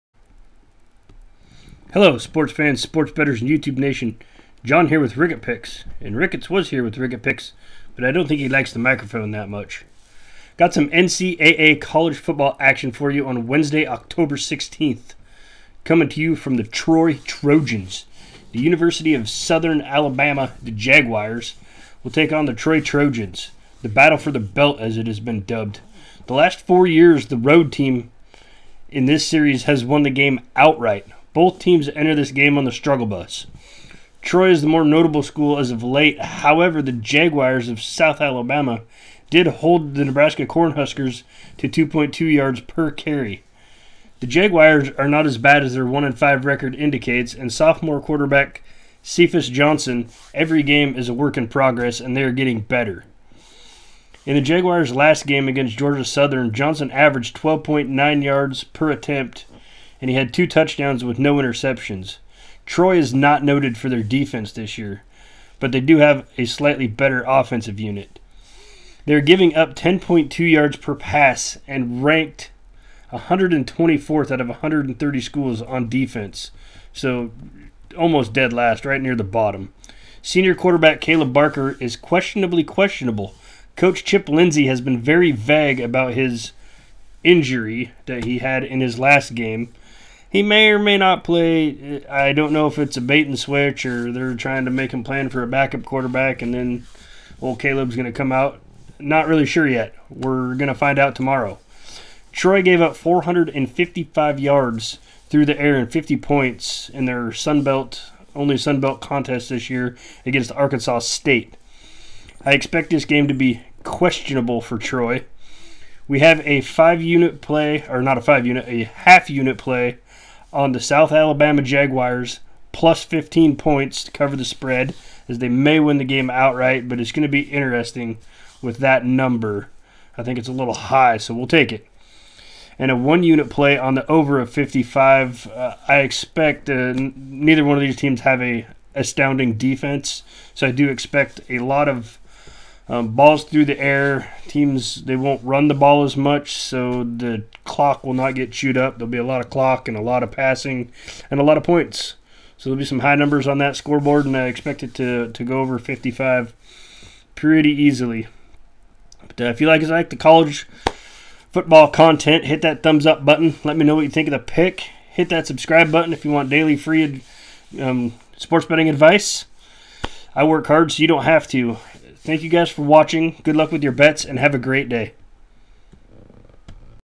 NCAA Football Free Pick, 10-16-19, University of South Alabama Jaguars @ Troy Trojans
Daily sports betting advice. NCAA Football, 10-16-19, University of South Alabama @ Troy Trojans. Lets crush the books.